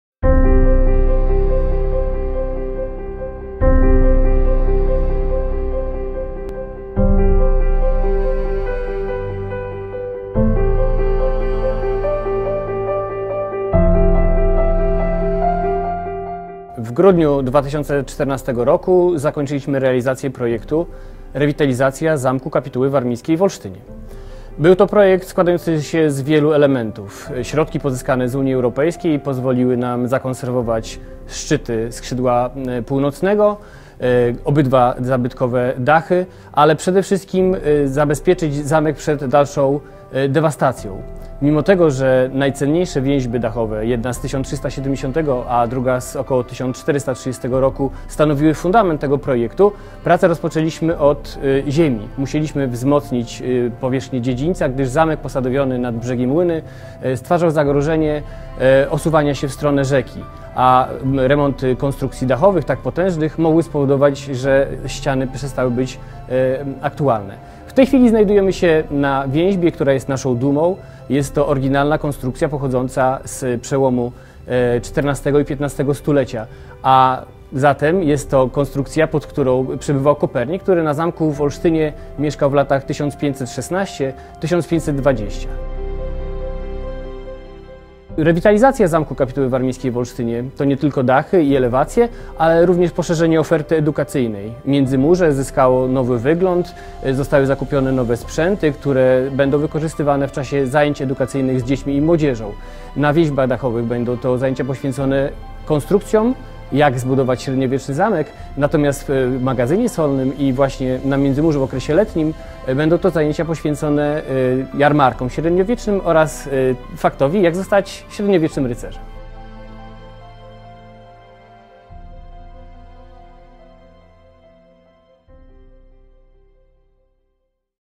Zamek w nowej odsłonie
Film prezentuje projekt "Rewitalizacja zamku kapituły warmińskiej w Olsztynie" zrealizowany przez Muzeum Warmii i Mazur w Olsztynie
w ramach RPO WiM. Realizacja jest częścią multimedialnej wersji biuletynu Warmia i Mazury.